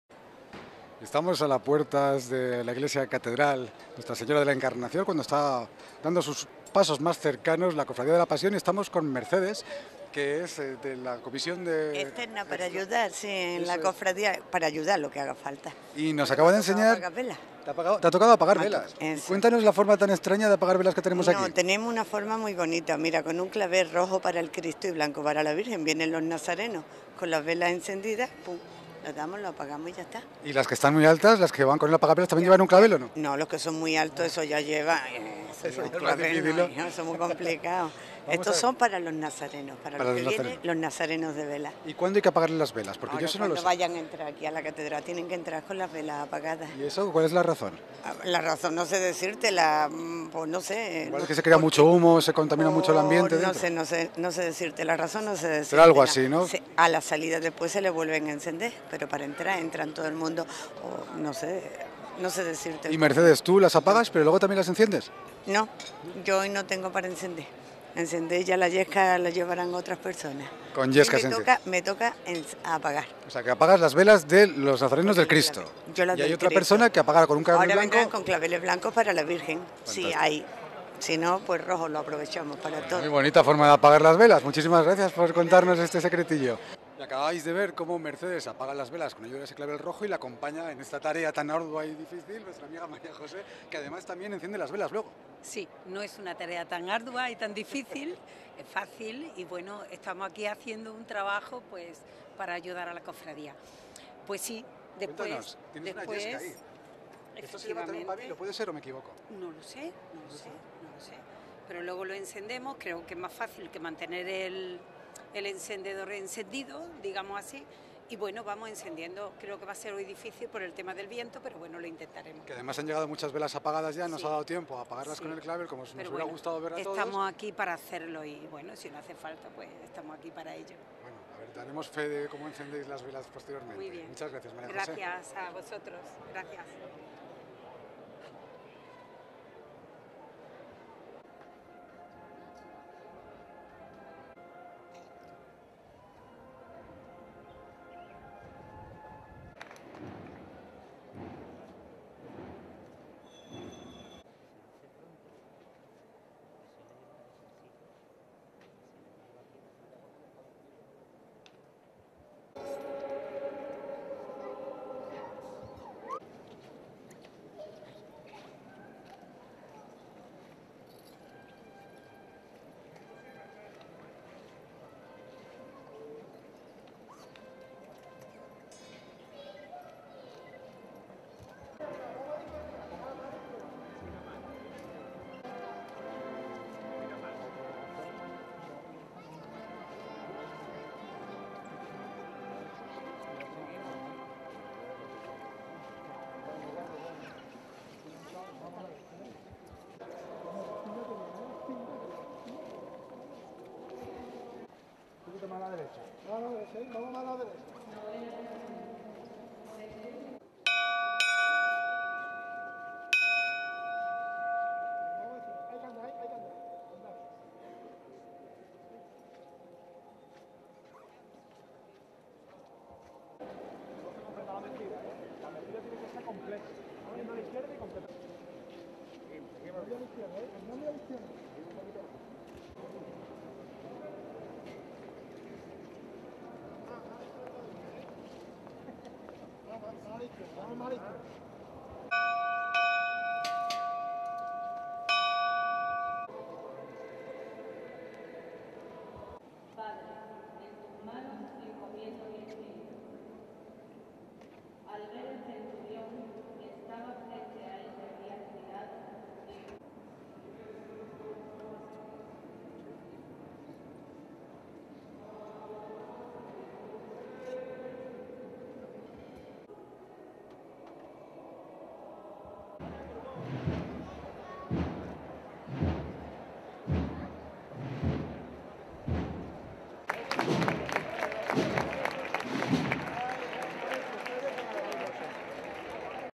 La Cofradía de Pasión en la Catedral (Lunes Santo 2018)
Los tronos de Jesús Nazareno de Pasión y María Santísima del Amor Doloroso (Cofradía de Pasión) ingresan en la S.I. Catedral